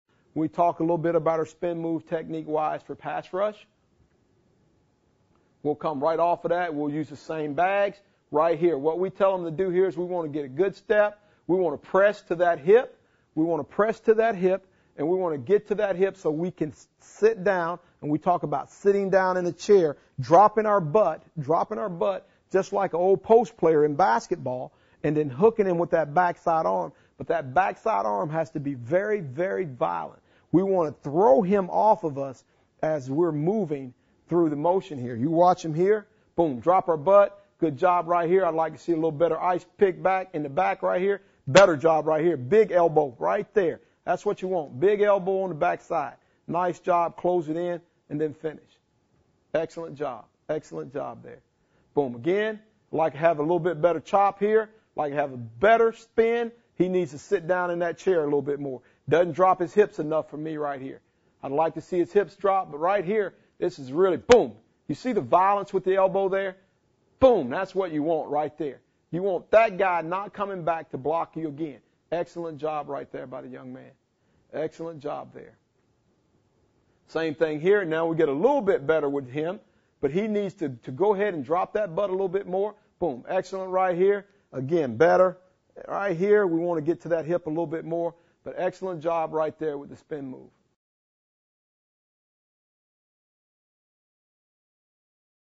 Aggressive Drills for Defensive Linemen
In this DVD presentation LSU Defensive Line Coach Brick Haley offers a great balance of fundamentals and techniques for run and pass situations. Haley walks you through each drill and incorporates game footage from LSU's 2009 season, when his defense finished 11th in the nation in points allowed.